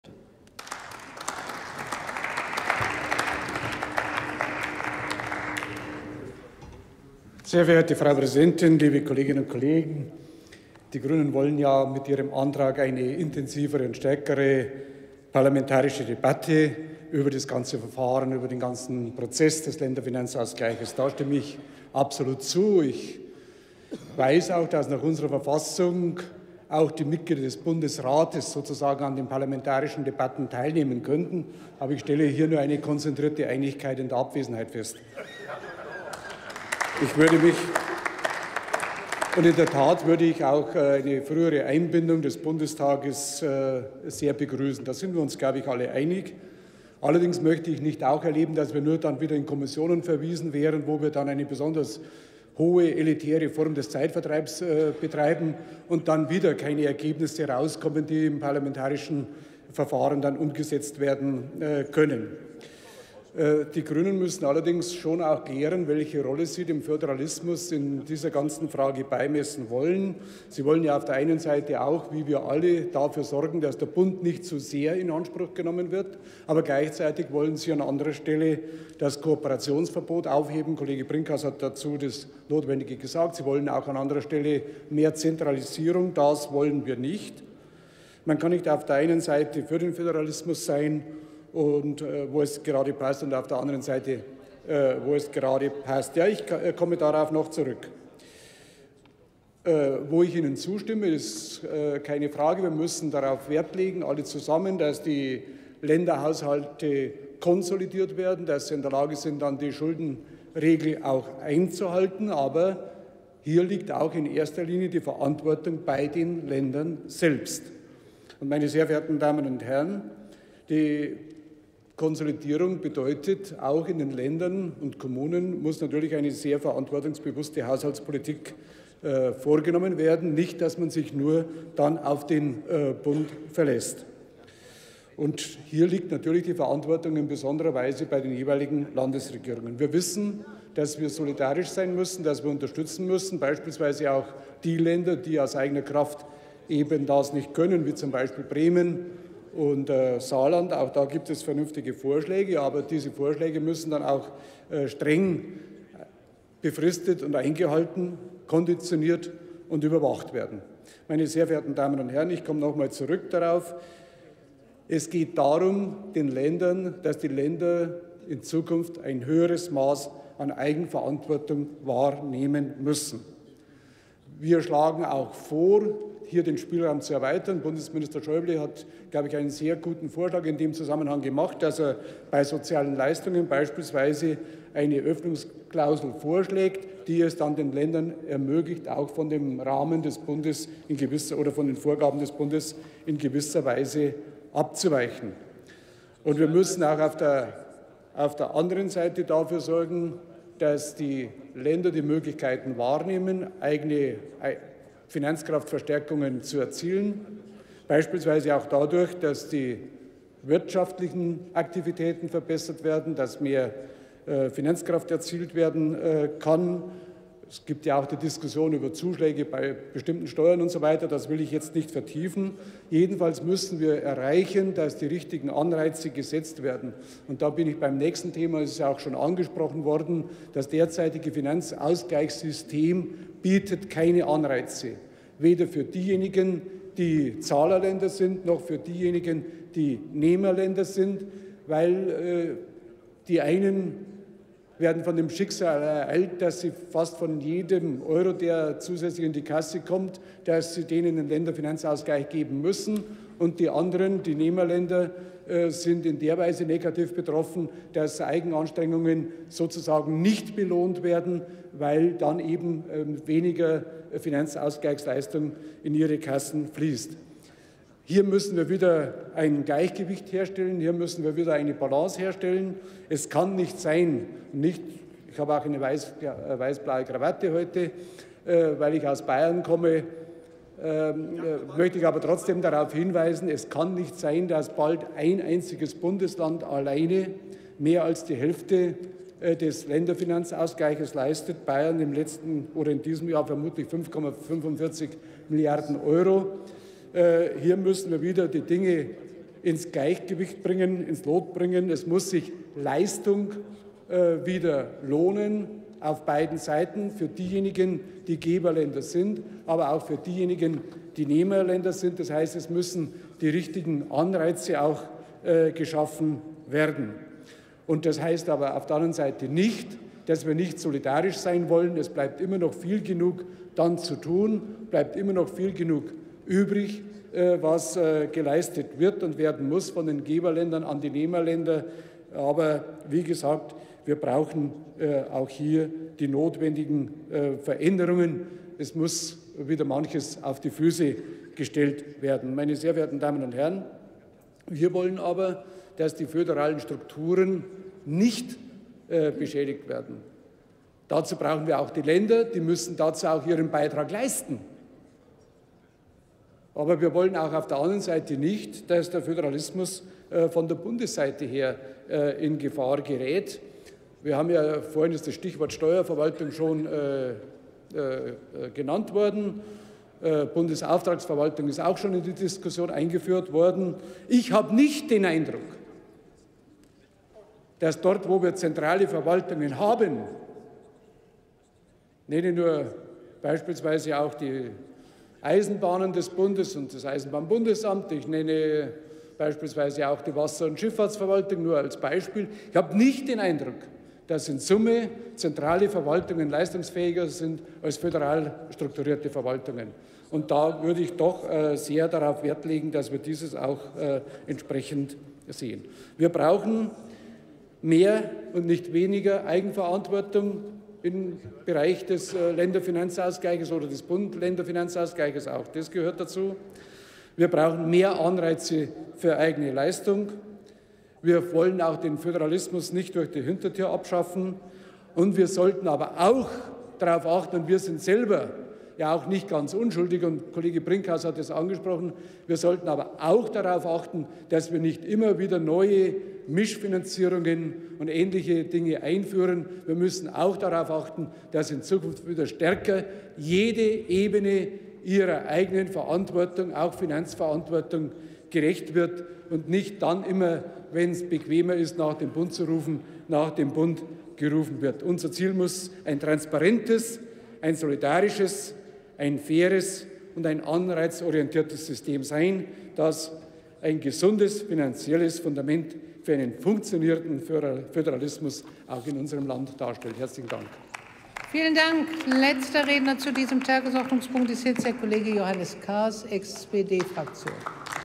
Bartholomäus Kalb: Neuordnung der Bund-Länder-Finanzbeziehungen [Bundestag 13.05.2016]
Die Neuordnung der Bund-Länder-Finanzbeziehungen jetzt angehen
Drucksache 18/8079

18. Deutscher Bundestag
171. Plenarsitzung
Tagesordnungspunkt: Neuordnung der Bund-Länder-Finanzbeziehungen
Redner: Kalb, Bartholomäus, CDU/CSU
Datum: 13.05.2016
Plenarprotokoll:
UVID:6833961